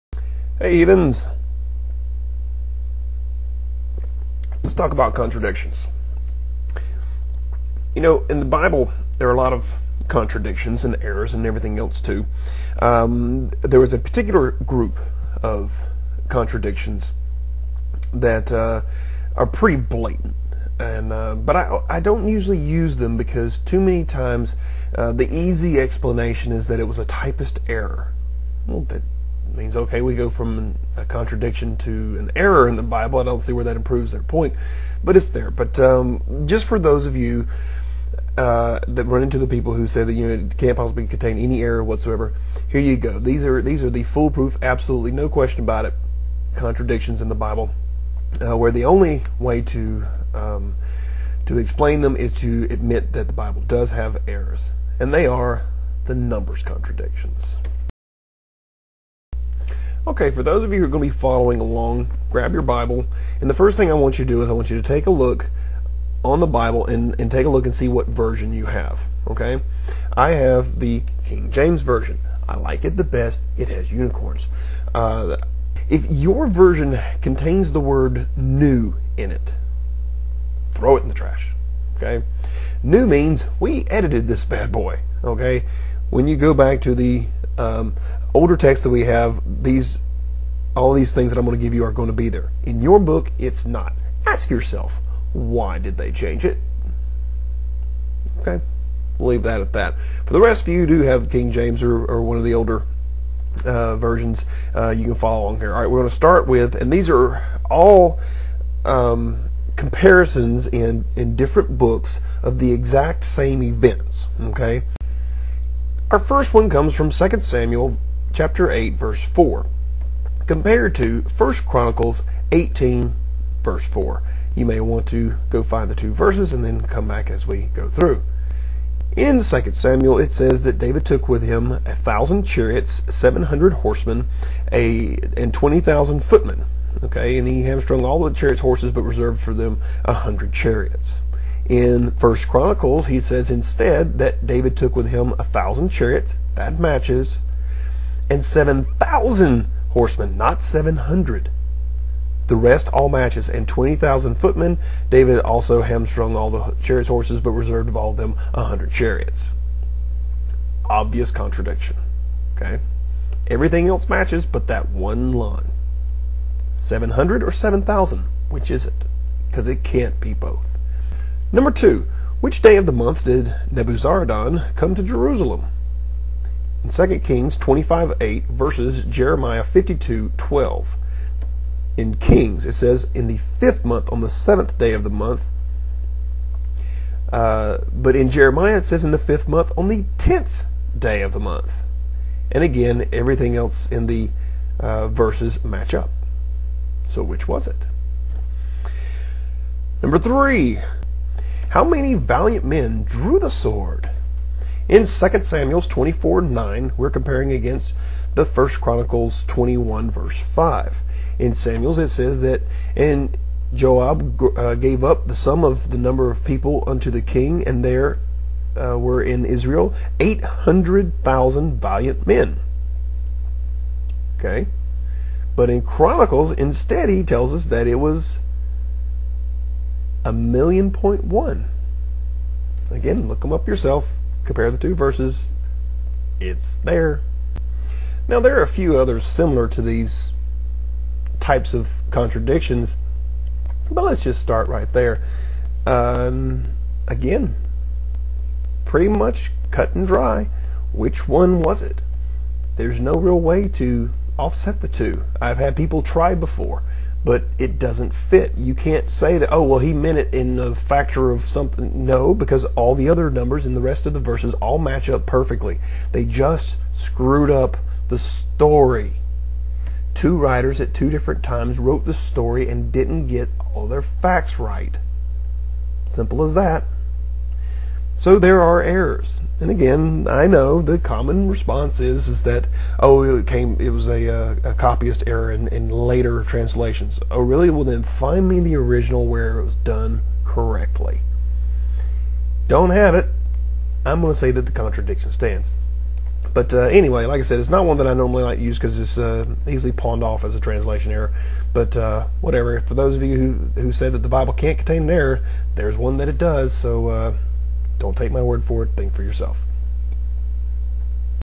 The Number Contradictions
You can't find a clearer contradiction than two verses telling the same story with two different numbers in the story...

Yet I don't like using these...but as a favor to a request to cover them...here they are: